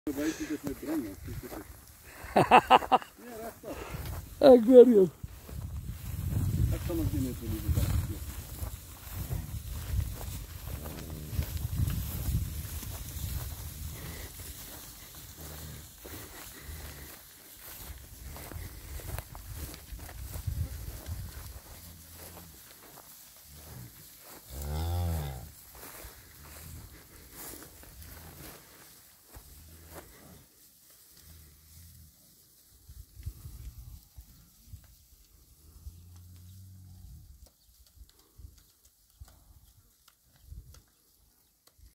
72+72 x koeie met kalwers
72+72 x koeie met kalwers . Mpumalanga.  omgewing.  Beefmaster bulle loop heeltyd tussen koeie. Geen ou koeie. BM skoon. Koeie weeg 550-580kg gemid en kalwers 150kg gemid geskat. R18950 + btw.